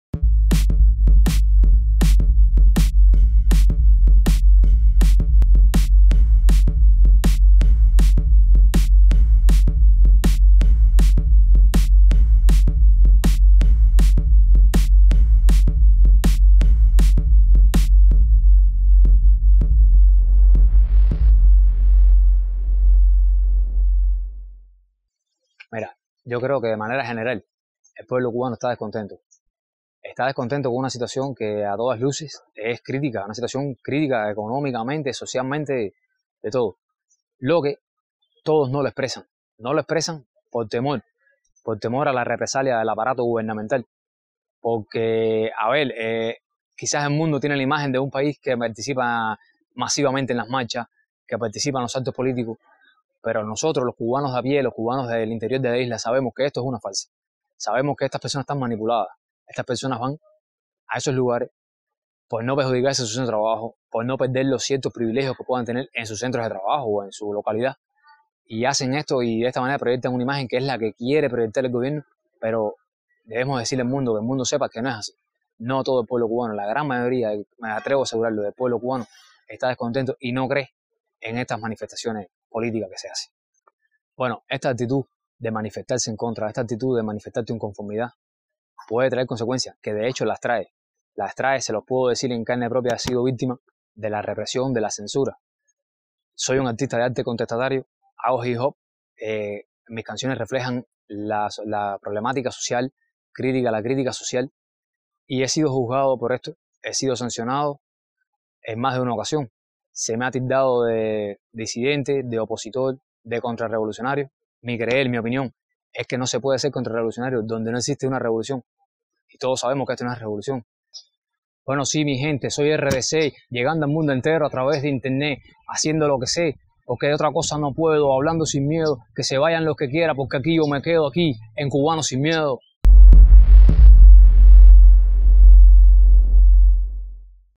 Cubanos sin miedos: Roberto Díaz Cue
El artista independiente Roberto Díaz Cue nos habla de la censura en Cuba y del descontento que existe entre la gran mayoría de cubanos.